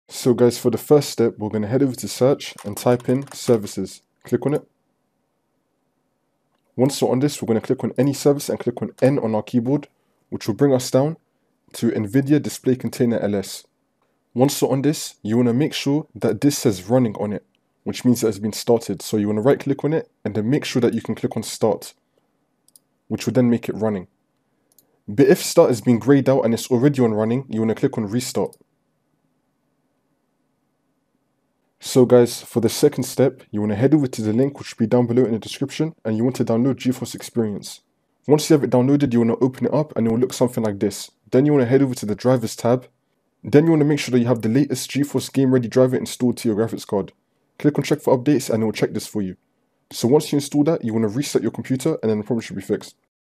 How To Fix NVIDIA Control Panel is Not Showing or Found in Windows 11
Here in this video tutorial, I will show you guys how to fix or solve NVIDIA Control Panel is not showing or missing in your Windows 11 Desktop Under Right click Menu option.  This also works on Windows 10 PC & Laptop.